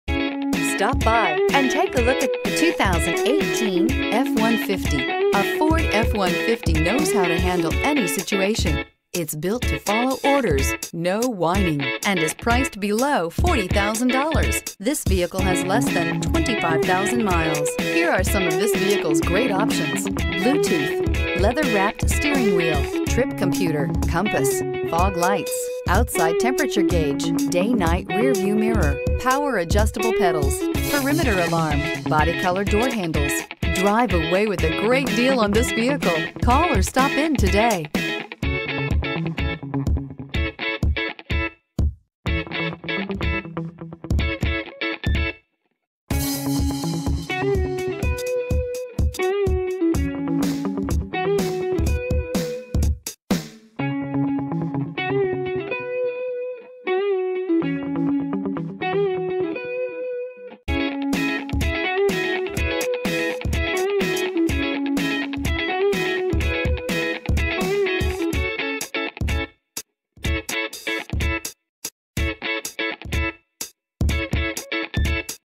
2018 Ford F-150 Port Richey FL PPD71063
2018 Ford F-150 XL

11613 U.S. Highway 19 N

Clean CARFAX.brWhite 2018 Ford F-150 CUSTOM LIFT KIT RWD 10-Speed Automatic 2.7L V6 EcoBoostbrbr20/26 City/Highway MPGbrbrbrAll Trades Accepted! Come experience the LO-key approach to car sales! Guaranteed Credit Approval!